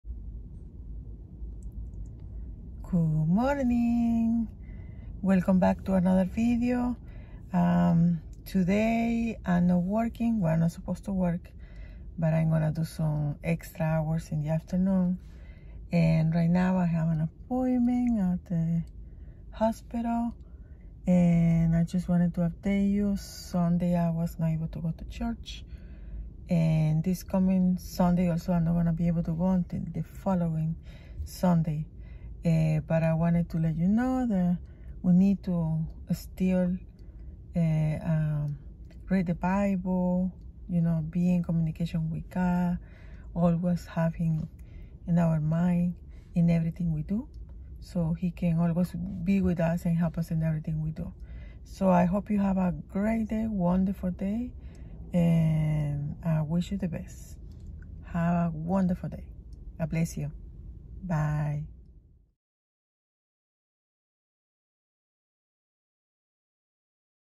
🌸 Updating Day to go to Hospital 🏥 🌷🌷 Everything is Ok 💖 beautifullife Thankful BelieveinGod 🌸 💖
Good morning 😃

Today I wanted to update you because on Sunday I was not able to go to church ⛪️ but we still need to have communication with God even do He always is with us.

Xoxo 💋